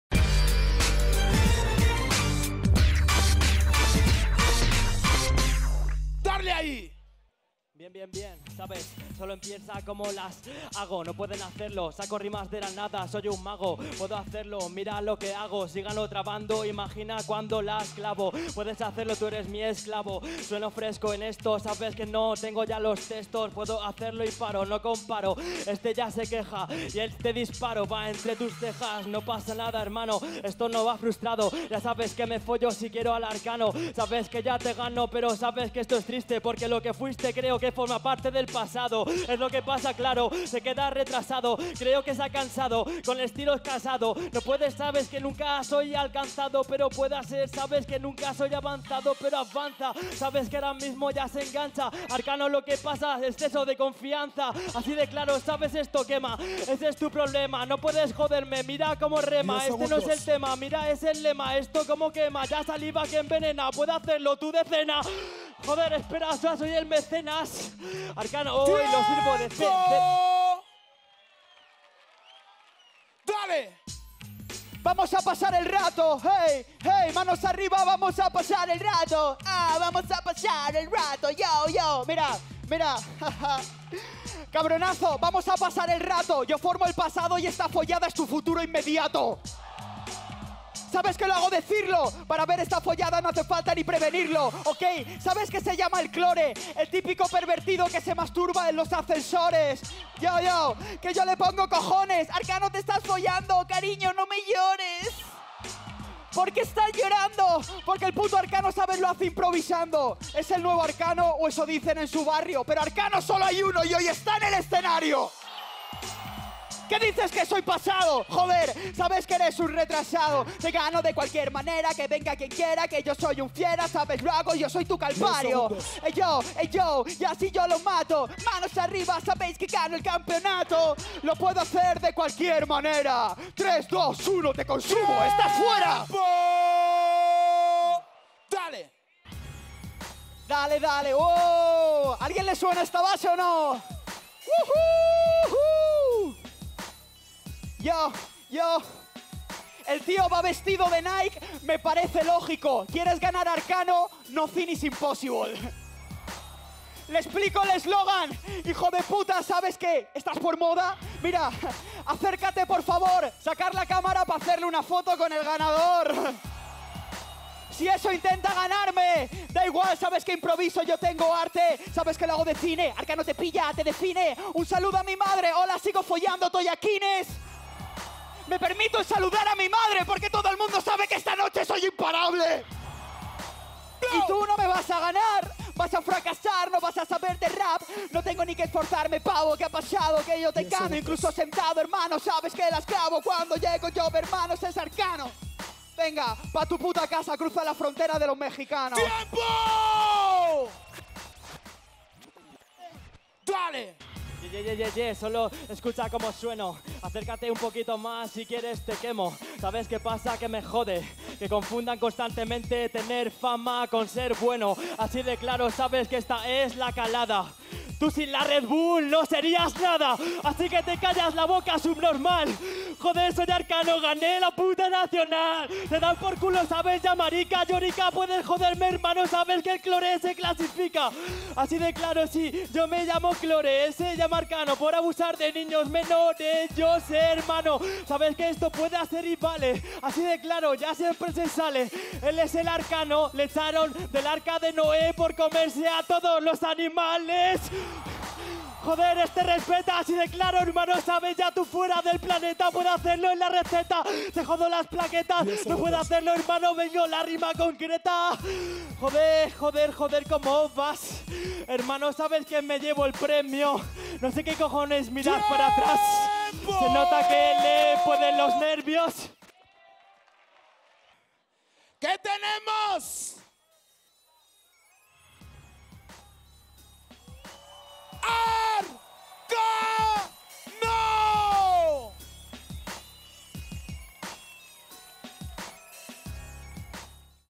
Arkano vs Klore - Semifinales - Semifinal Madrid - Red Bull Batalla de los Gallos 2014 (Oficial)
Nos hemos cambiado de canal, a partir de ahora todas las batallas se van a subir